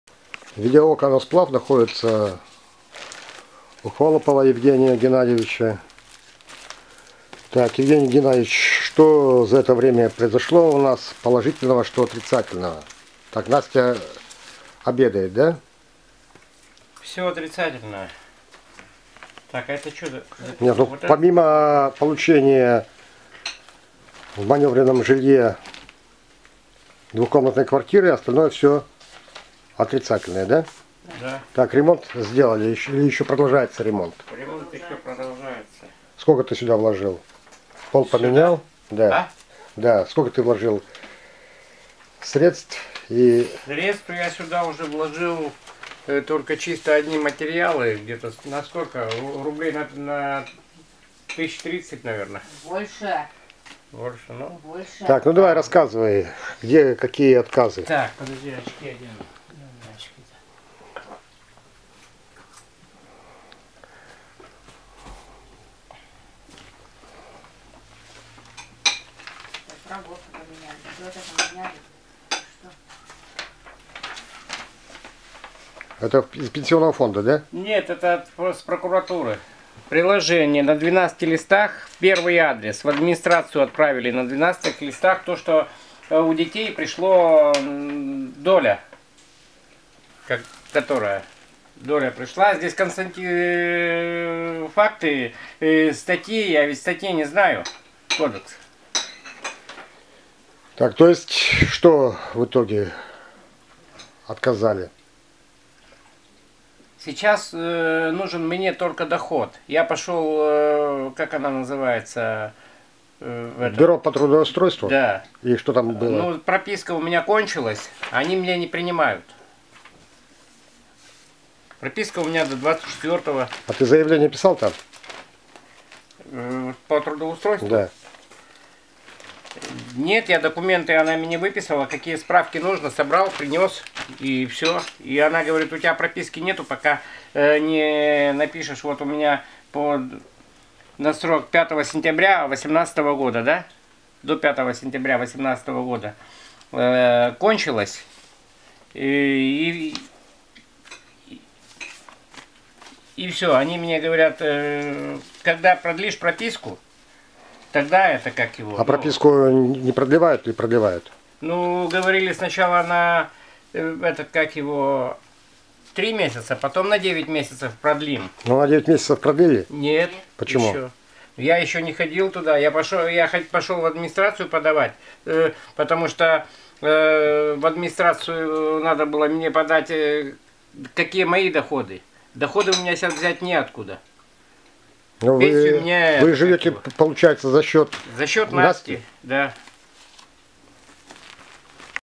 "заМОРочка", у Евгения Геннадиевича // 12 12 2018
"заМОРочка",  у  Евгения Геннадиевича Холопова, "живём   за счёт Насти",  в семье взрослых 2+3 детей (1 -DS^ ДЦП - родовая травма), г.  Сыктывкар, Республика Коми, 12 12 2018